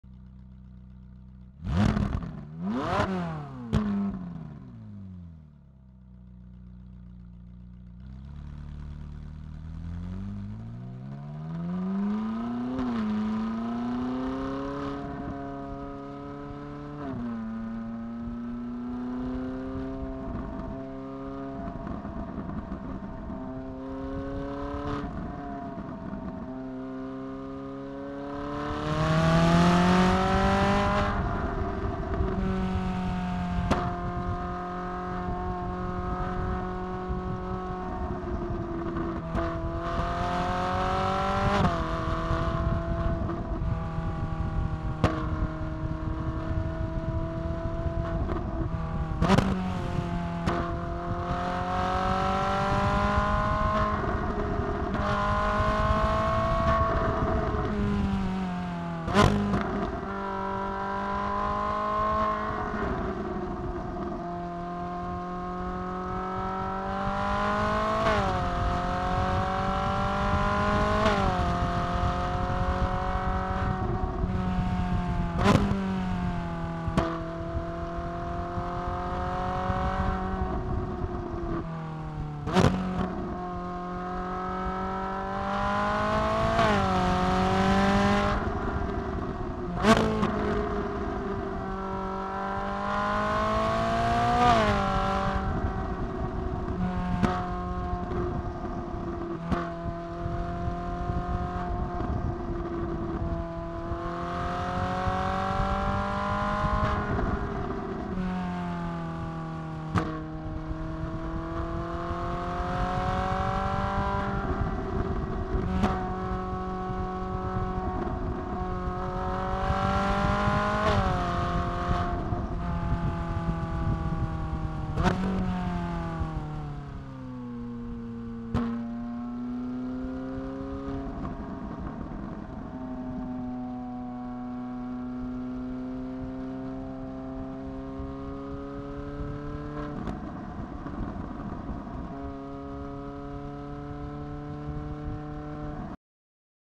Ferrari 458 Italia - testing audio
Just testing a microphone setup - all driven under posted speed limits